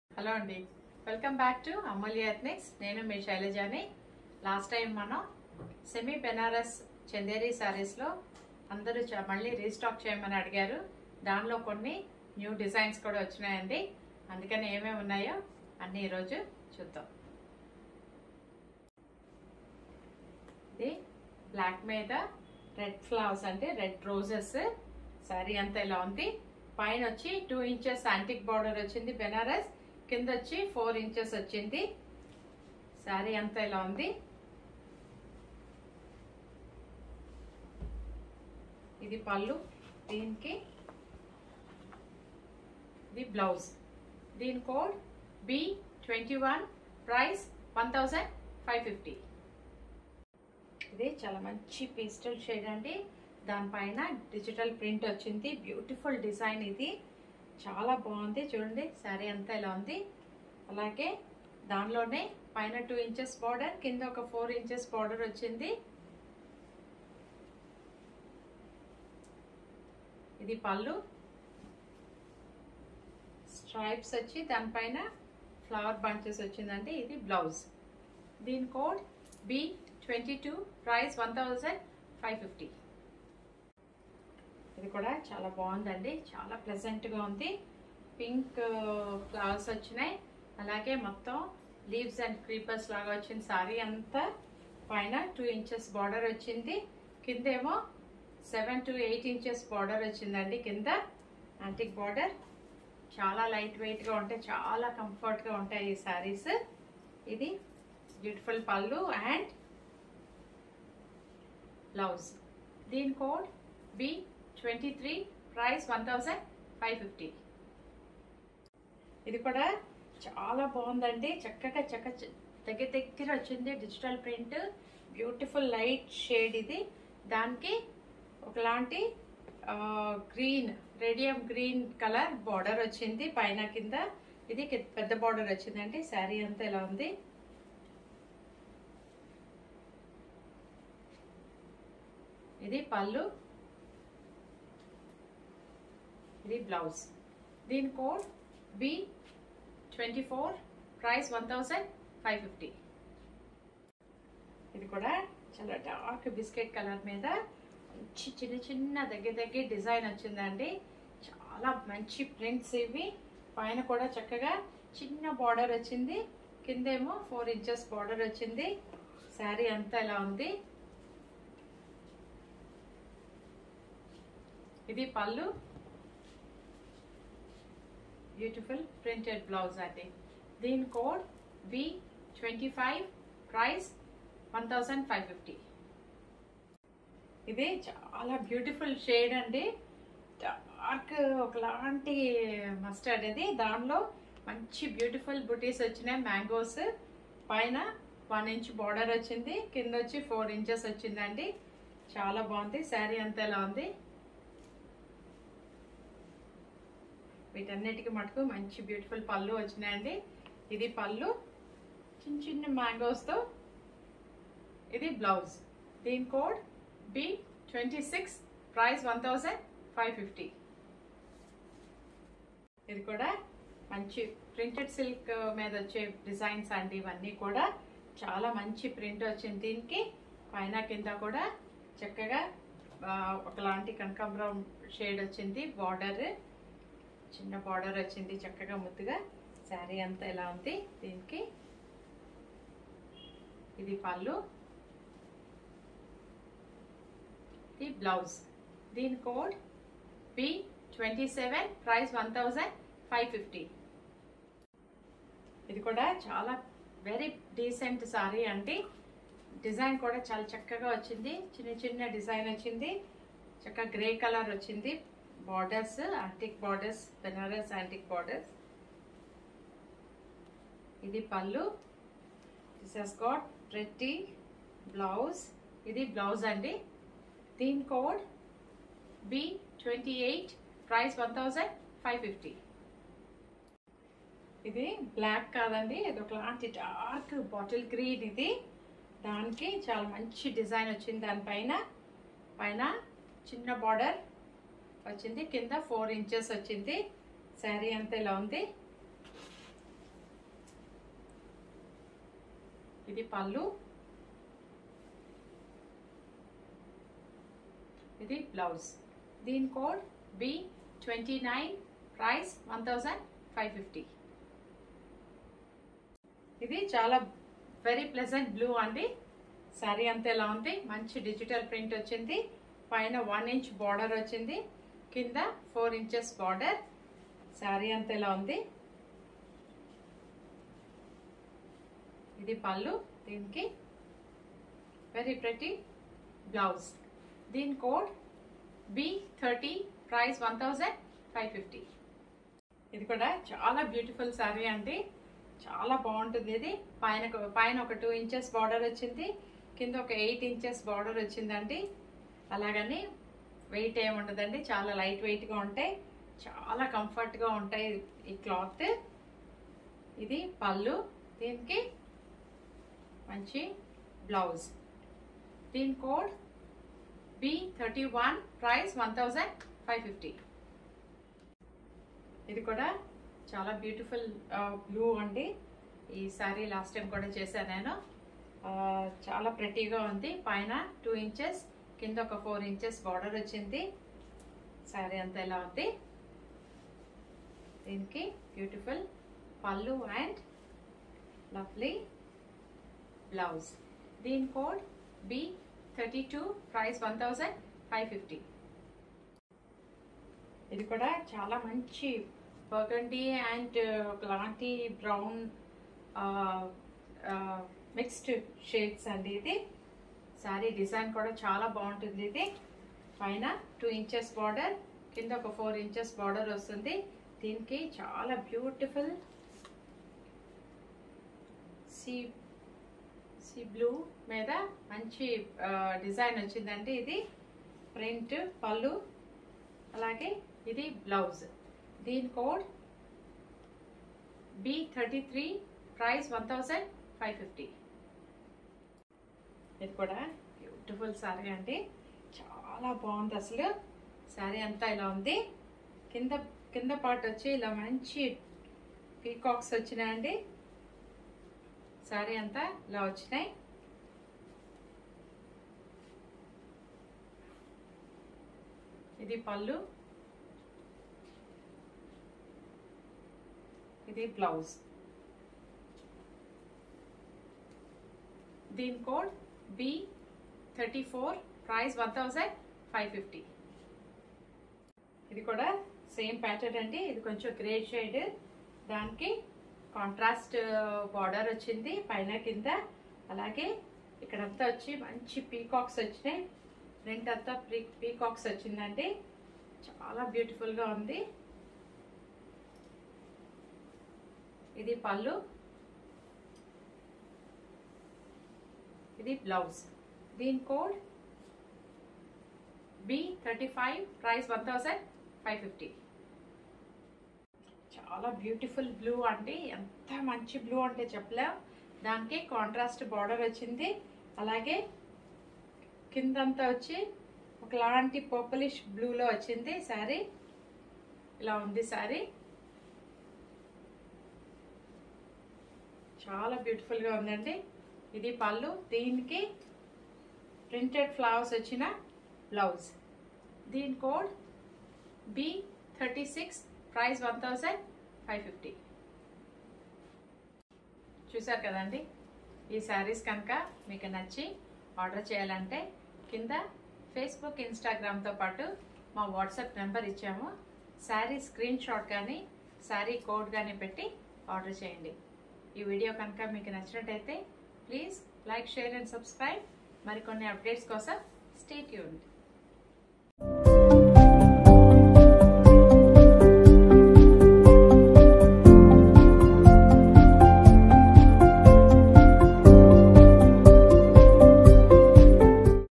Semi Chanderi Banaras Sarees | Rs.1,550 | July 11th 2021
Our customers loved the charm of Chanderi with the antique Banarasi borders. These sarees are an ode to the culture of central India. Lovely and light floral prints with elegant gold-toned Banarasi borders add just the right amount of festivity to these royal Chanderi sarees.